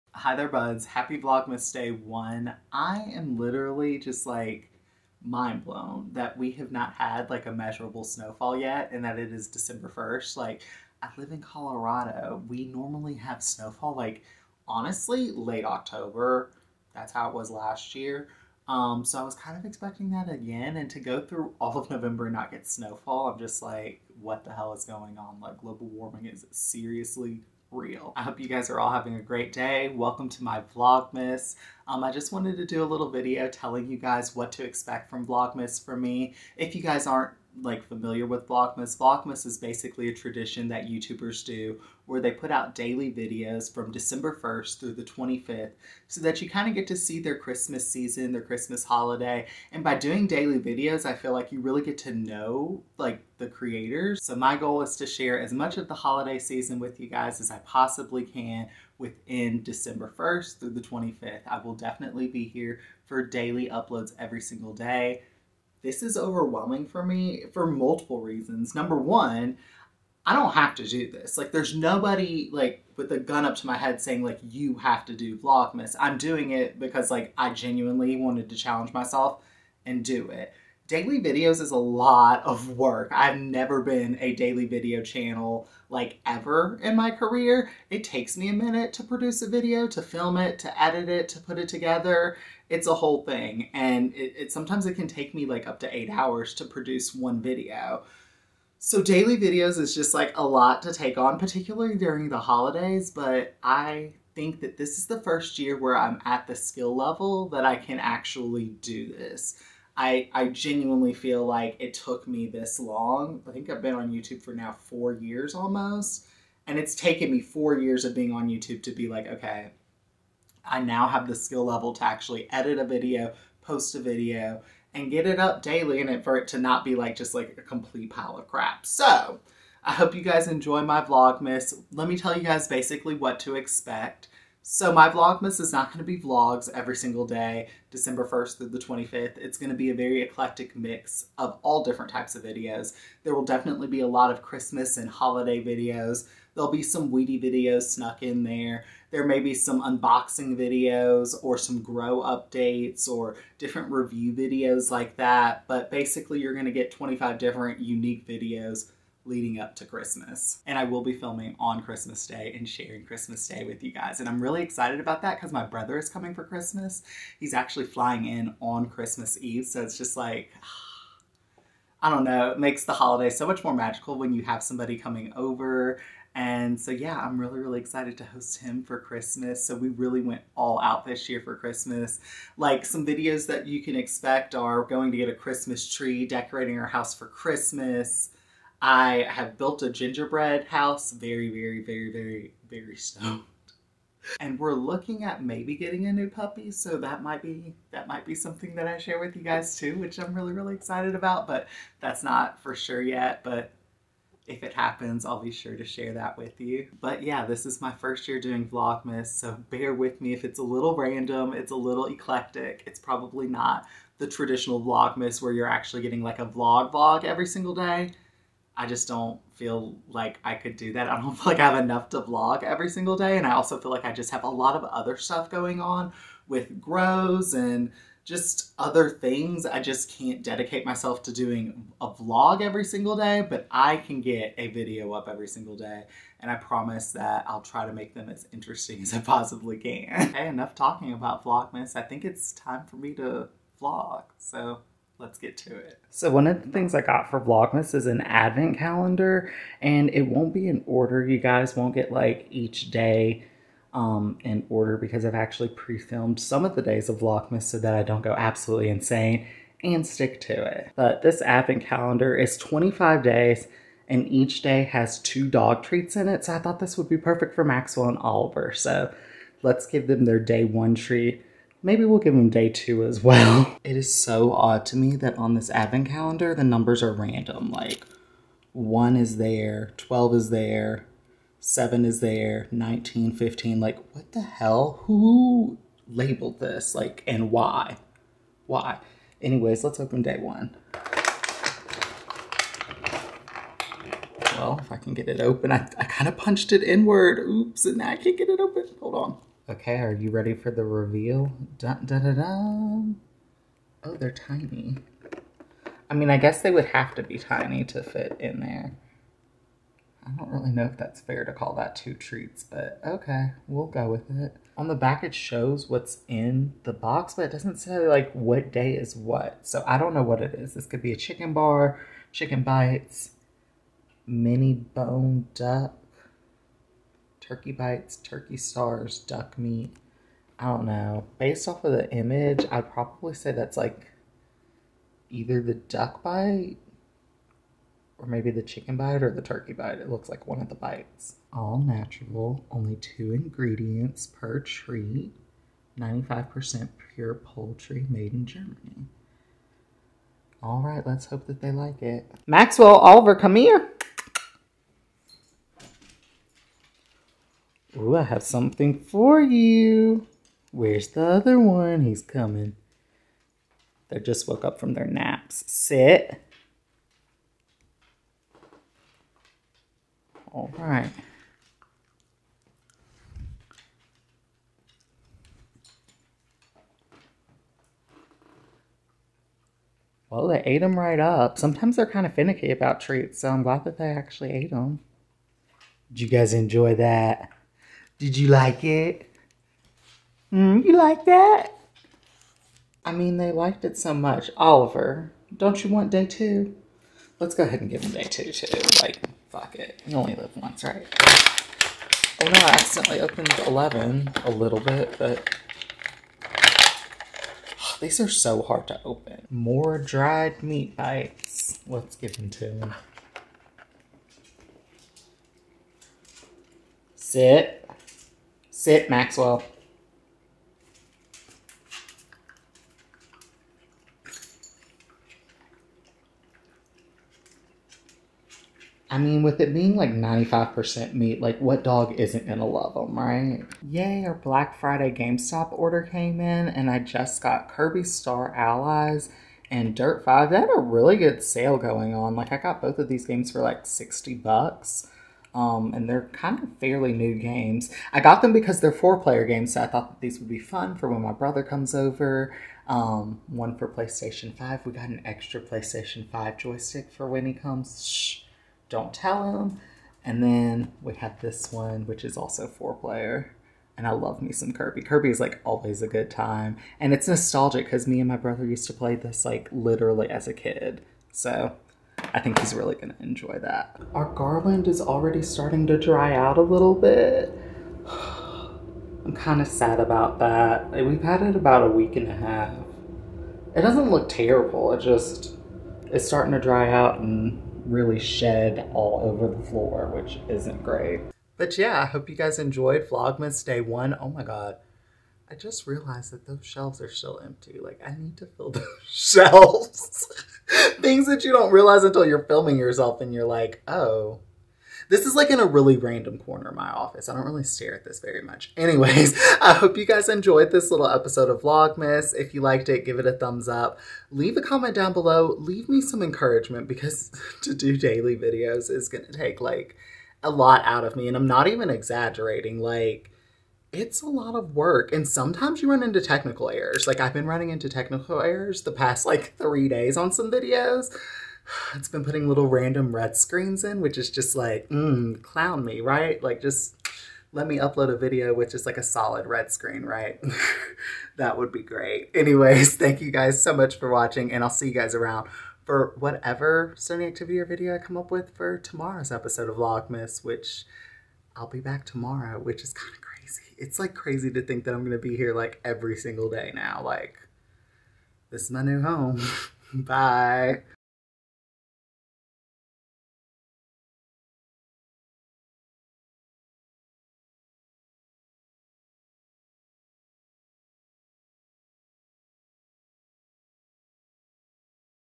VLOGMAS DAY 1!!!! // YUP I'M DOING 25 VIDEOS THIS MONTH!
Happy Vlogmas Day 1! I can't believe I'm doing this. 25 videos in one month.  Leave some encouragement in the comment section. Hope to celebrate the holidays with you everyday until the 25th :)